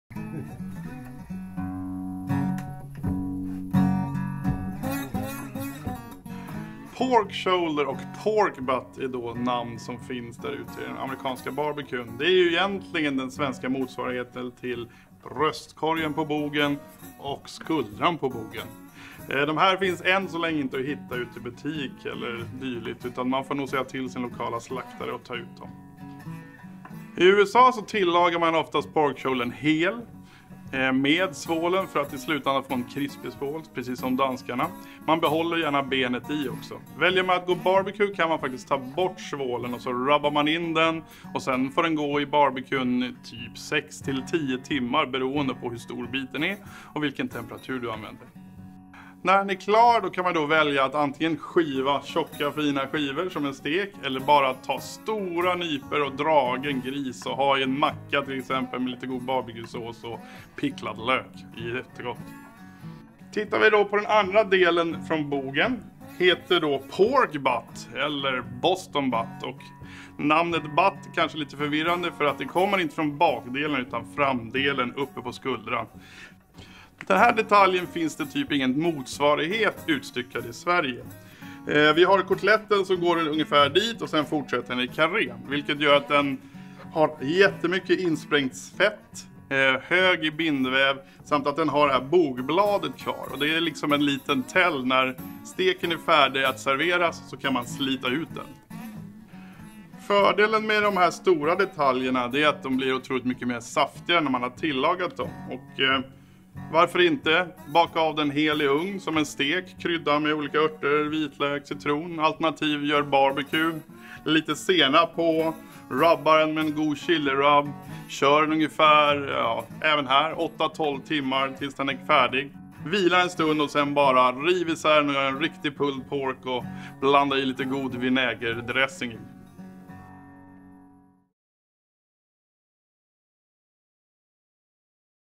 Amerikansk styckning på svenska, Griskött: Porkshoulder och Pork butt
Pork shoulder eller bogen är djurets bröstkorg och skuldra. Den styckas ofta olika i Sverige och USA. I Sverige säljs detta kött färskt eller rimmat, det färska ofta tärnat, strimlat eller skivat. Ibland hittar man också färdiga grillspett med kött från bog.

Pork shoulder är en stor detalj som i USA oftare används hel med eller utan ben i och den röks och lagas länge och serveras skivad eller dragen, pulled pork.

Pork Butt
Man skulle kunna tro att denna detalj kommer från grisens bakdel, men så är inte fallet. Pork Butt eller Boston Butt som den även kallas, kommer från den övre delen av bogen, närmare karrén och den styckningen har ingen riktig motsvarighet i Sverige. Den har ofta benet kvar och är full av bindväv och fett som smälter vid tillagning.

Tillagas hel i ugn eller som BBQ. Krydda med din egna favoritrub och tillaga på låg värme tills den är klar. God att göra pulled pork på alternativt kyla ner och tunnskiva och äta kall eller skiva tjockare och grilla på igen.